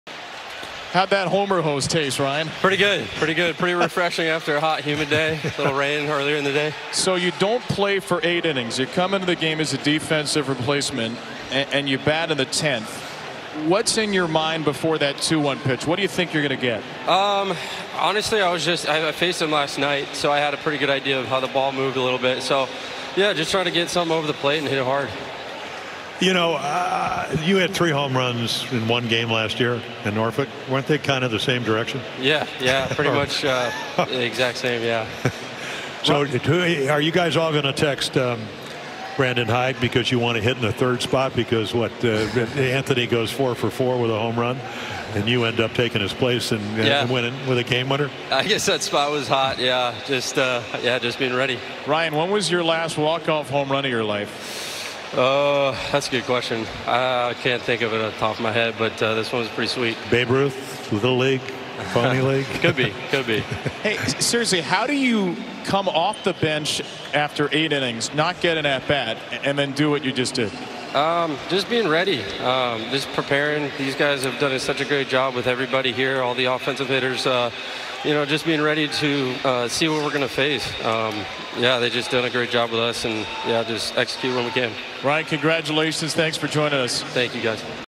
Ryan McKenna after his walk-off home run
Ryan McKenna joined the broadcast after ending the game with a walk-off home run.